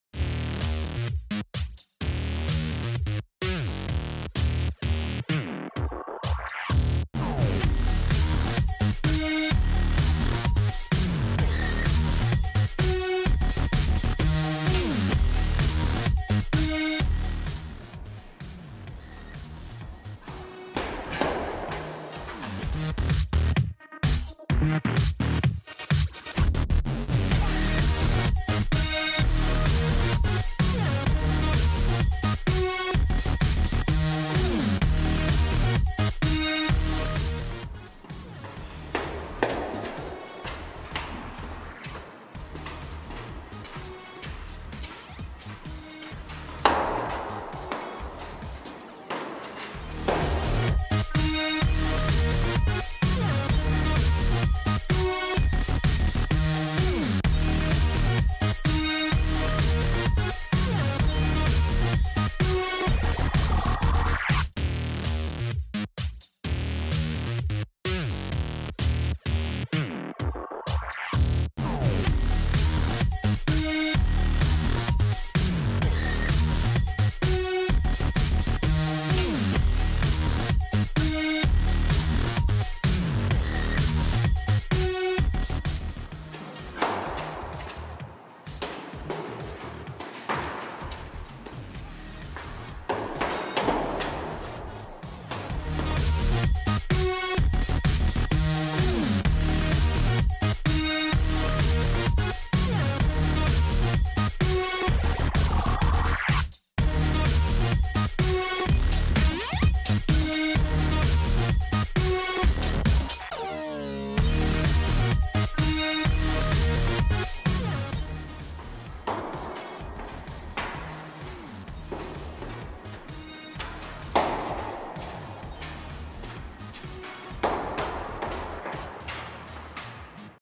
Phobic Miraging Seamless Juggle And Downward Pincher. Footbag Freestyle Trick
Phobic Miraging Seamless Juggle is a phairy set, counter spin, gyro mirage into seamless juggle.  Downward Garage is a different way to do a pincher, where you come at it from above, rather than Letting it fall into the V of your knee.